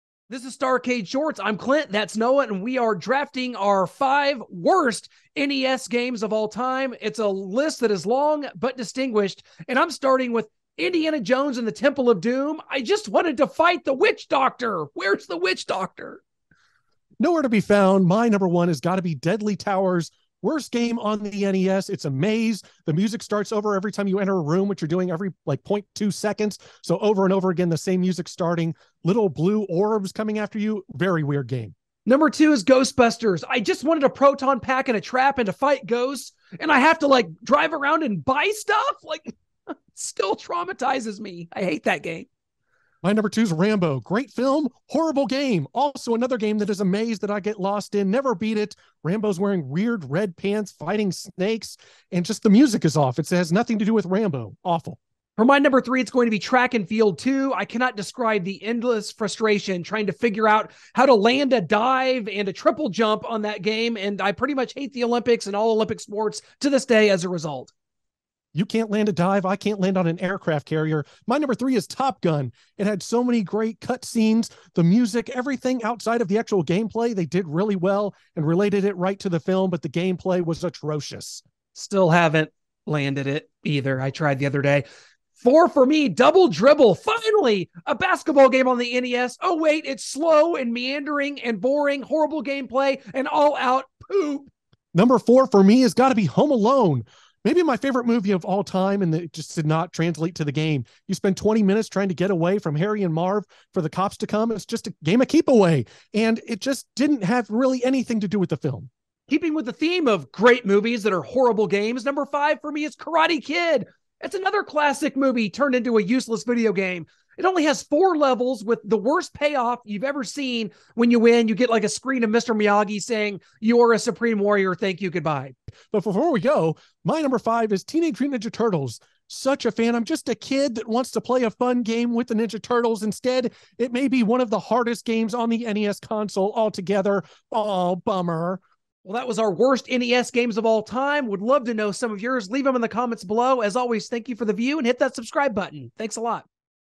Top 5 WORST NES Games Draft
Clint and Noah discuss their top 5 LEAST favorite NES games.